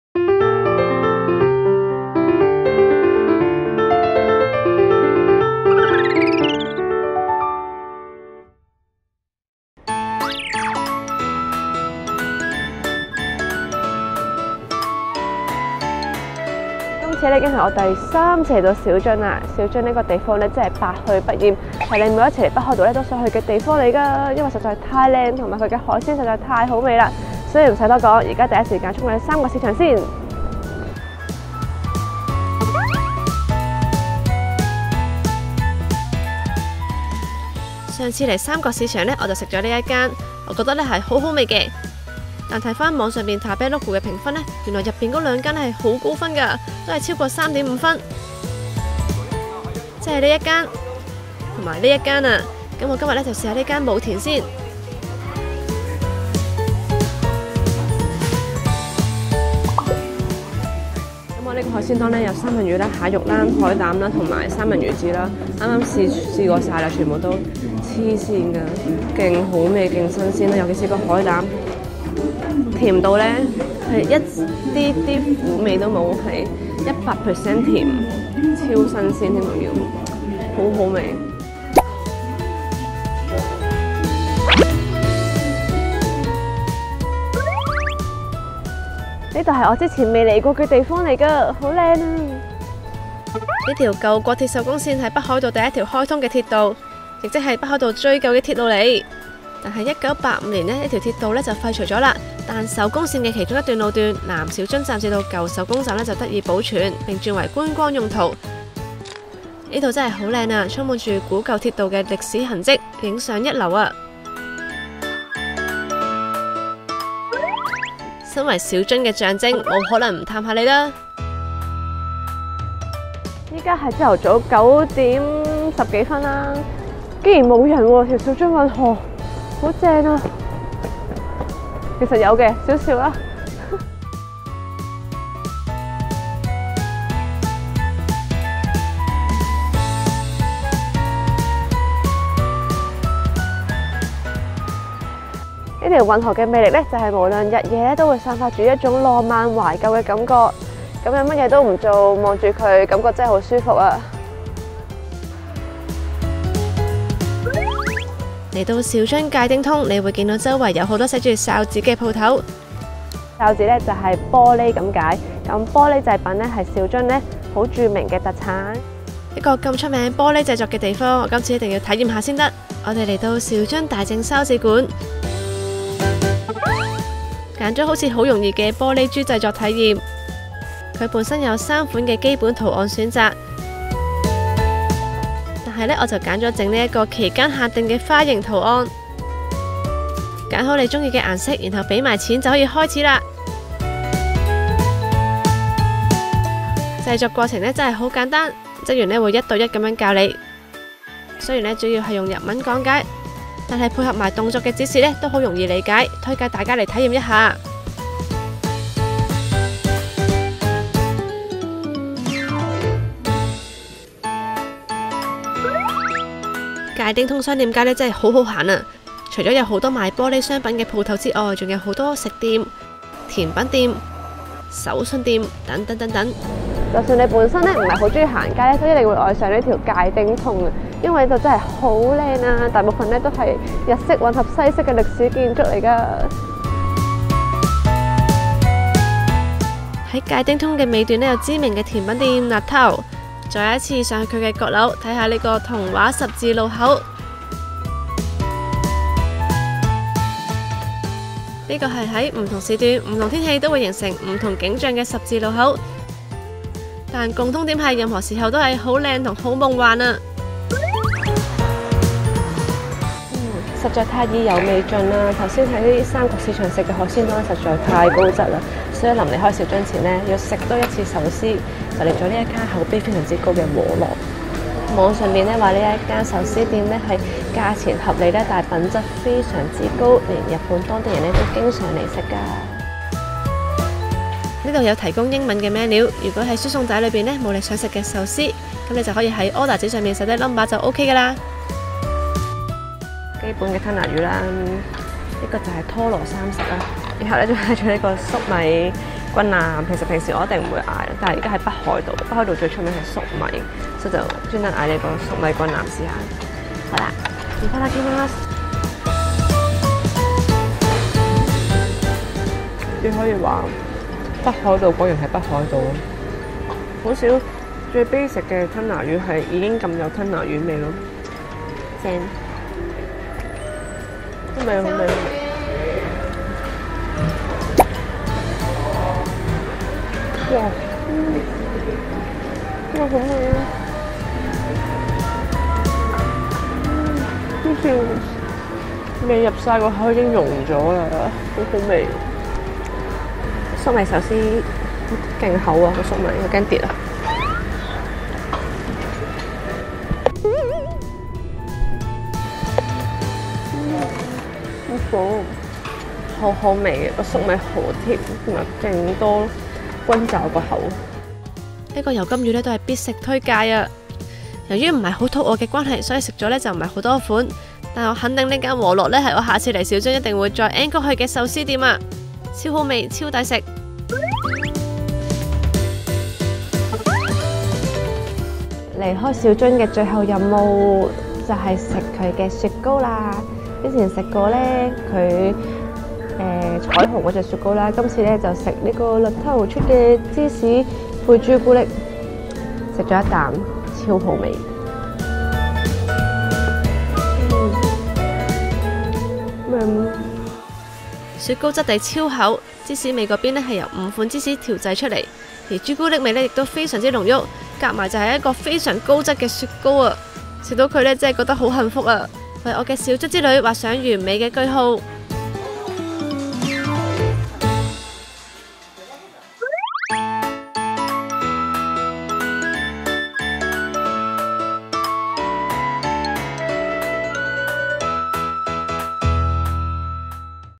【北海道鐵道遊】EP2 小樽篇（再去三角市場、舊國鐵手宮線、玻璃珠DIY、迴轉壽司和樂、LeTAO....）
►影片相關資訊
旅遊時期：2018年11月18日－11月22日
拍攝器材：SONY A7S＋Sony x3000 ＋CanonM50
剪片軟件：Adobe Premiere Pro

►多謝收看Thanks for watching!😆